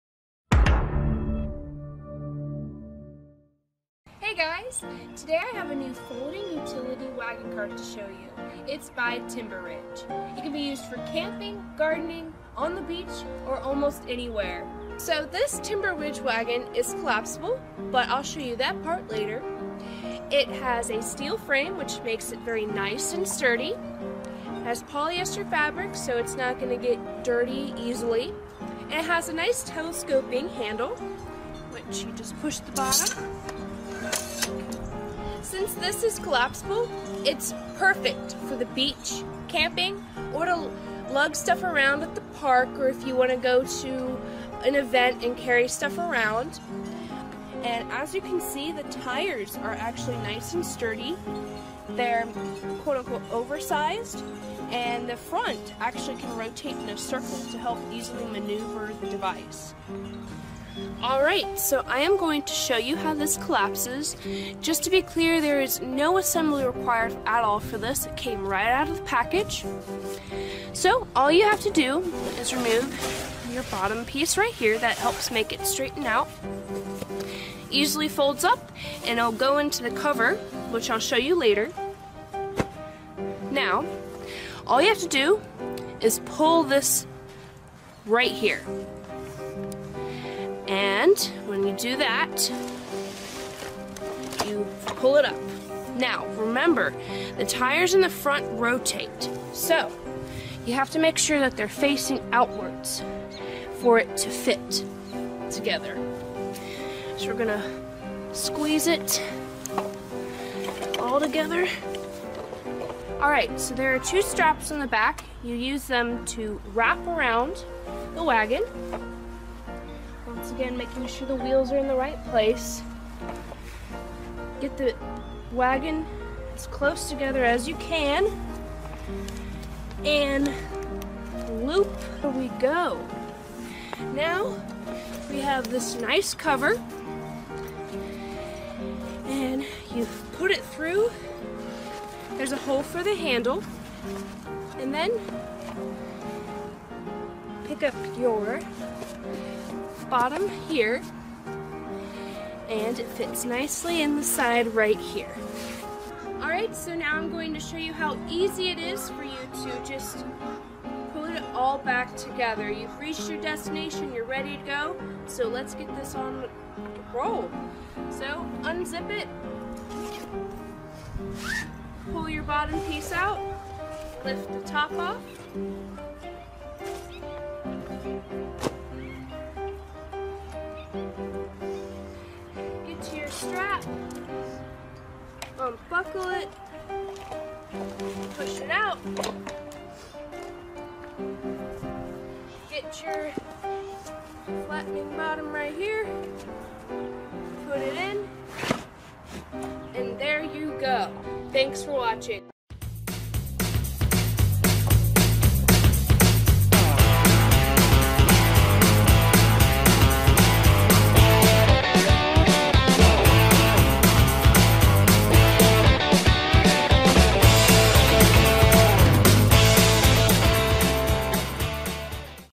✅ Mac Sports Collapsible Folding Outdoor Utility Wagon Review
Love this wagon. We have a rescued pitbull. He had been through many hurtful events . He struggles to walk long distance with the rest of our pack. He loves his wagon, and thinks while in his wagon he gets excited to be able to keep up with his brothers. Our pitbull is a therapy dog we take in his wagon.